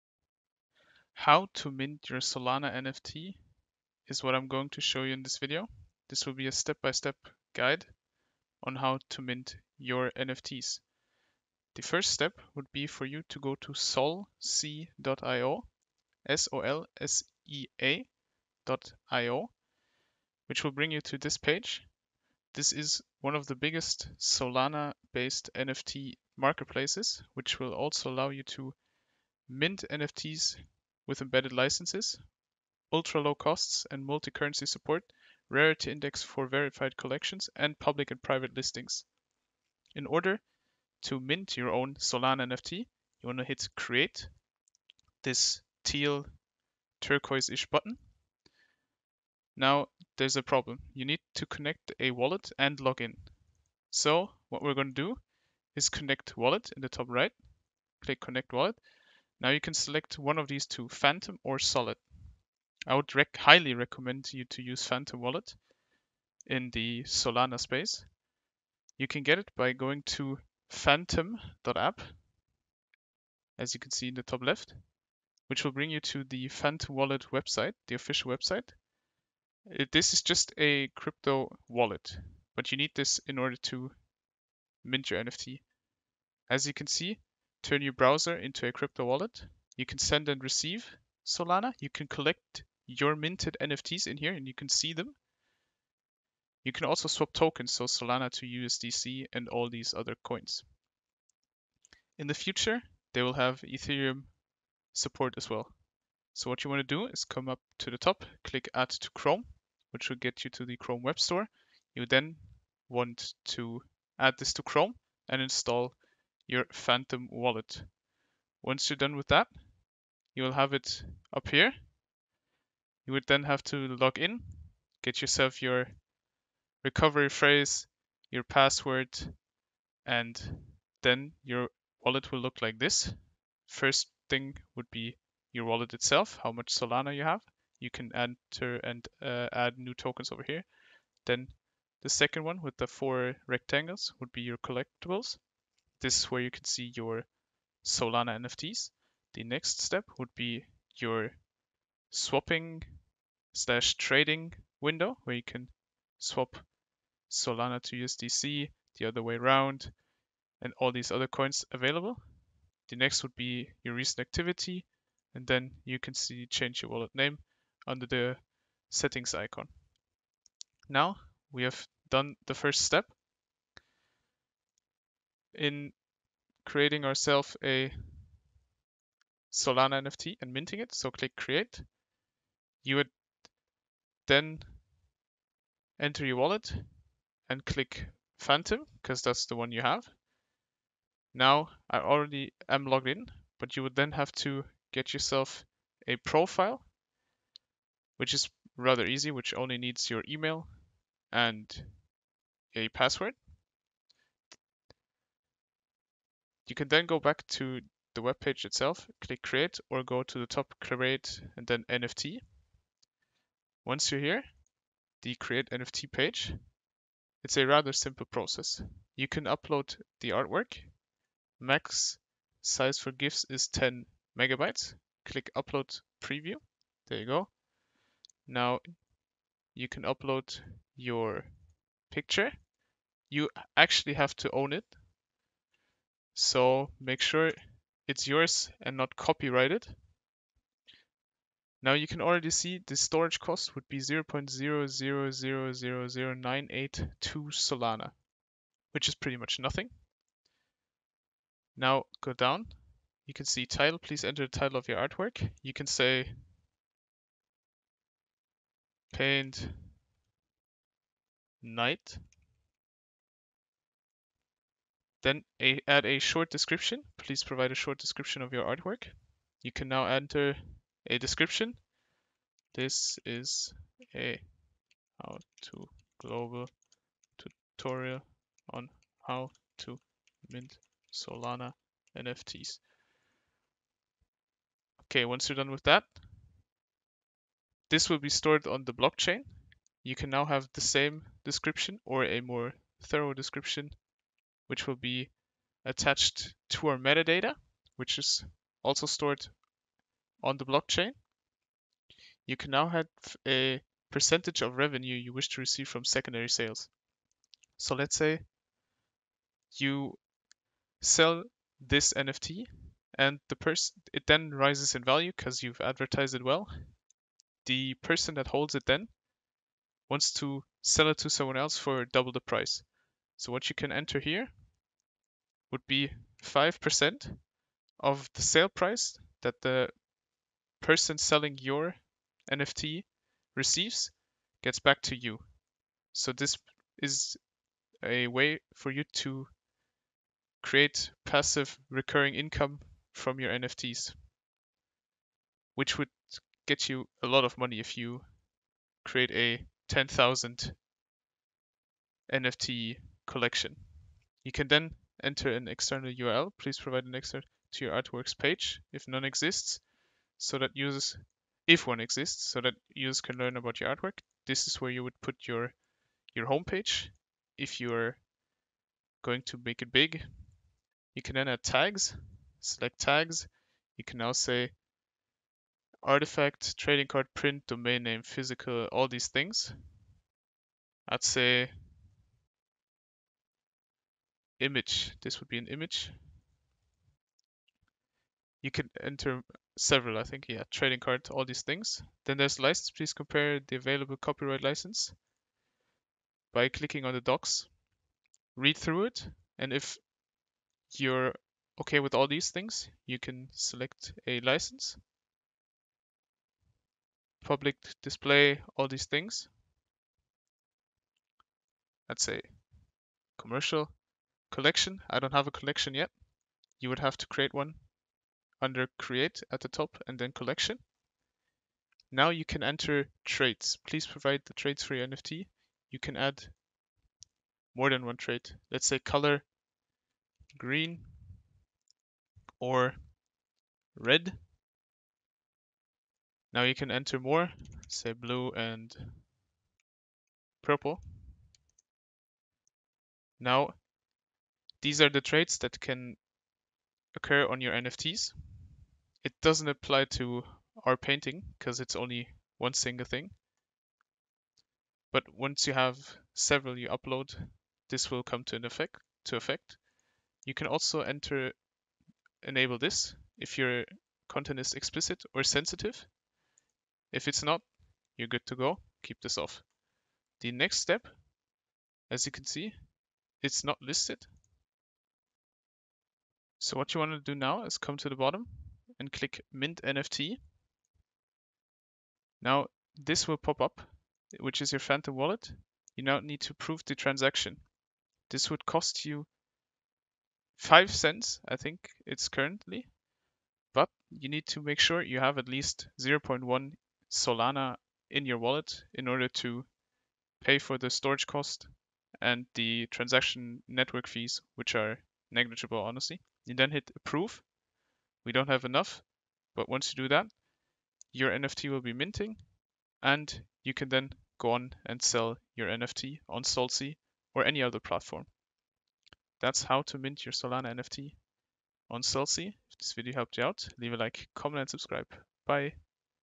How to MINT your SOLANA NFT (Step by Step)
This video shows you how you can mint your own art as a solana nft and sell it on any solana marketplace. Shown is how to make passive income with your nfts and cryptocurrency. Use the phantom wallet to buy and sell solana nfts.